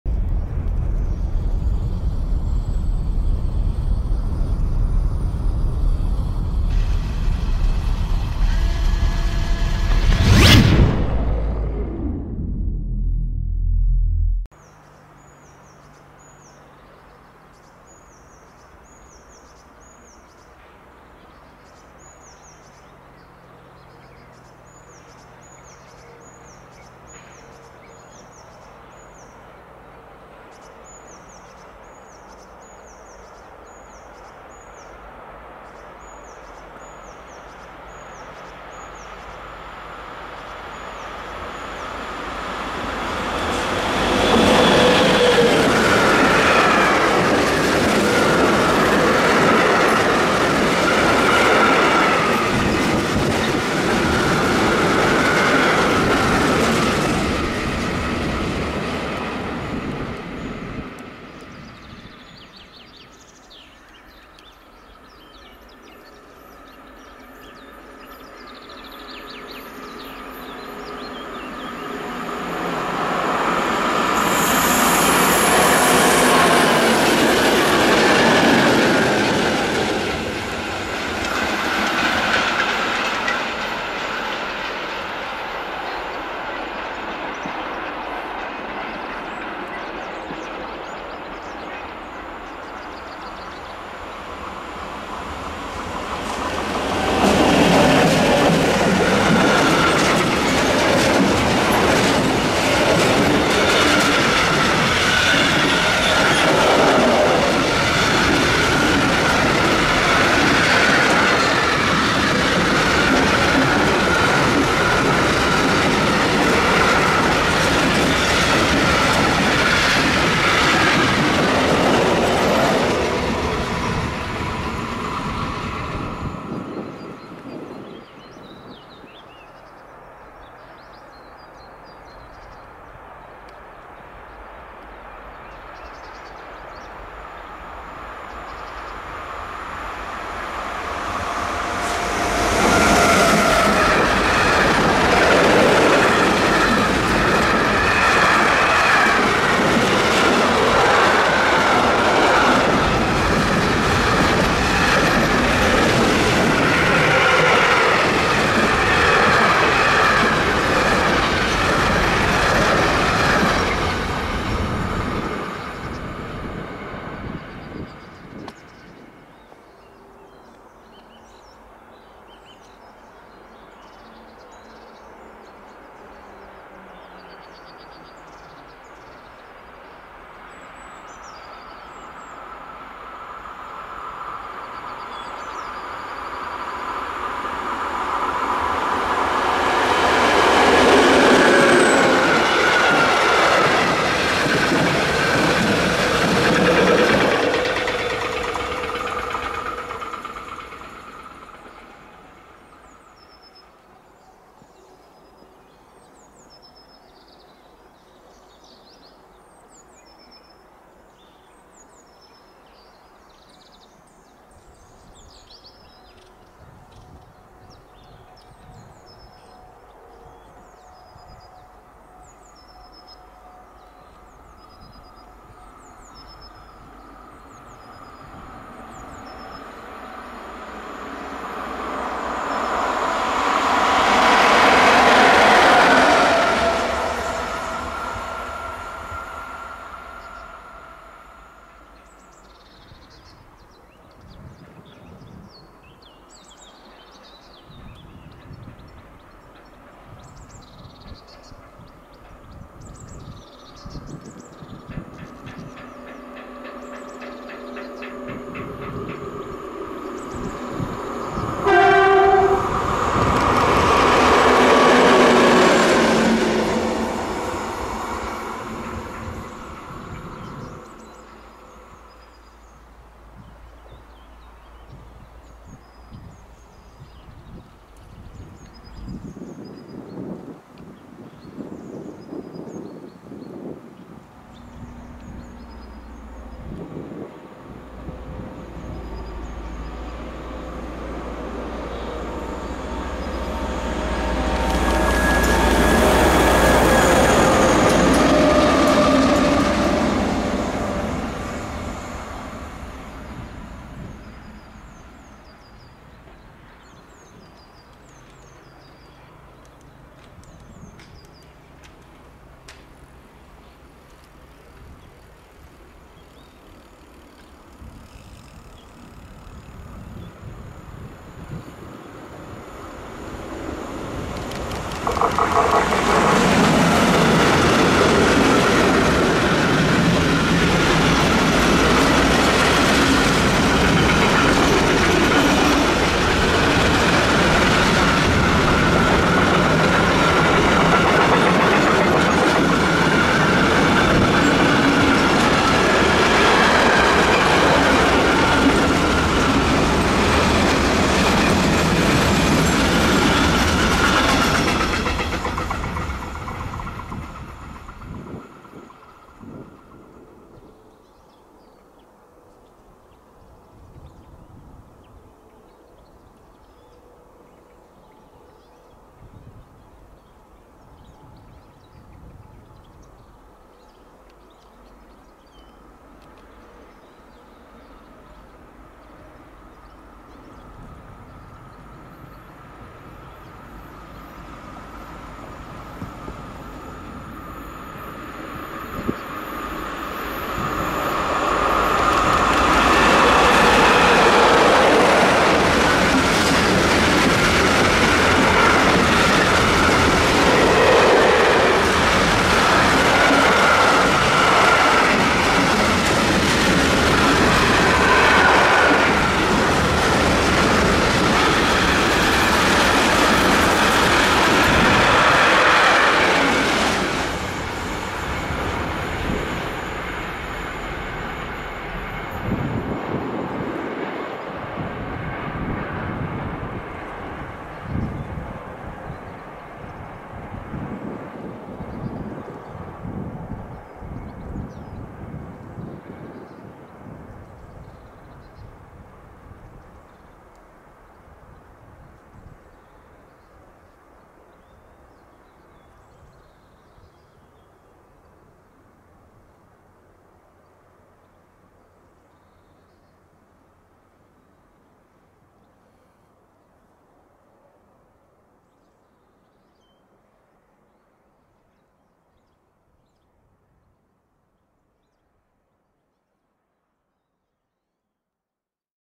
Viel Rot und eine Parallelfahrt in Saarmund 17.04.2020
Nach einer längeren Pause mal wieder ein Video aus Saarmund.
Zu sehen viel Rot und eine Parallelfahrt.
Viel Spaß beim Anschauen.
Gruß FH2010

Hello everybody!
After a longer break, here is a video from Saarmund.
To see a lot of red and a parallel ride.